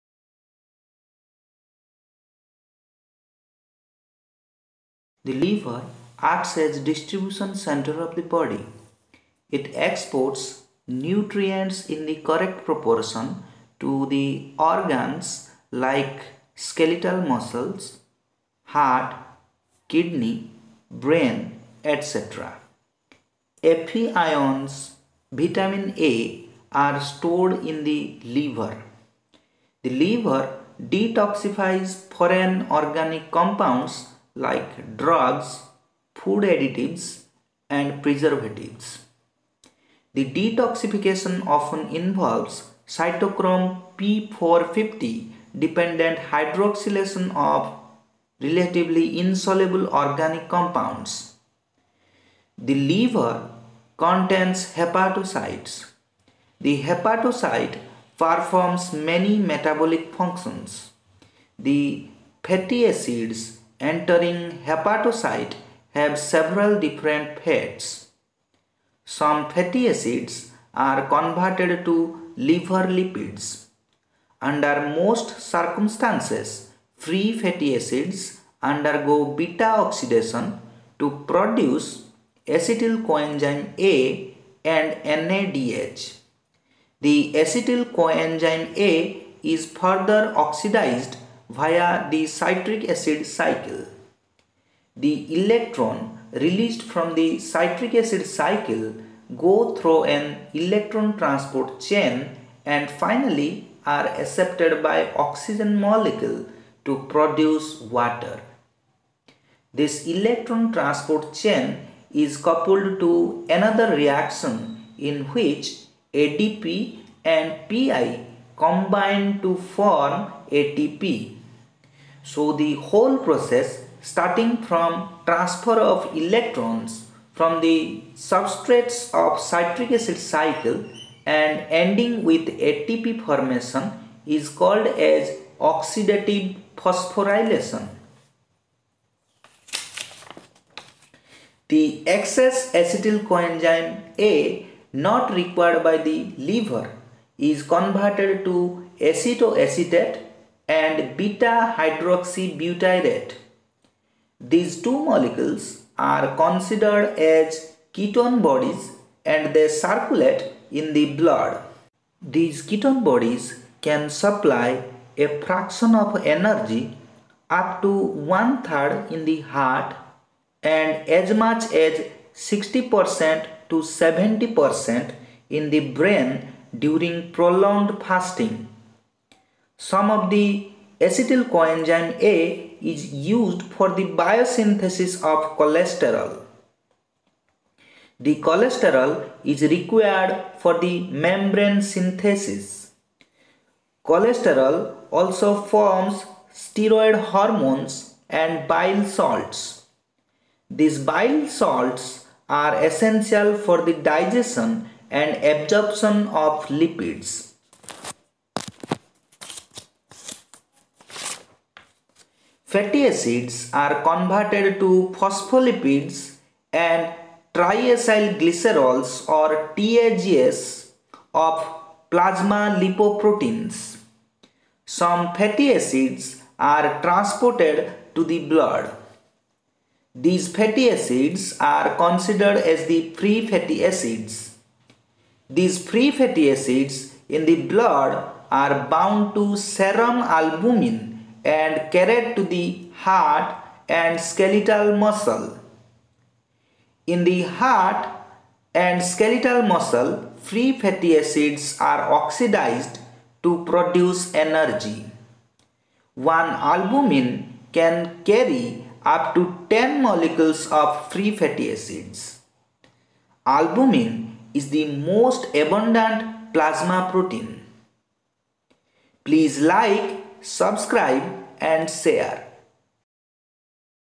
Fatty acid metabolism in liver
This video says about the metabolism of fatty acid in the hepatocyte of the liver.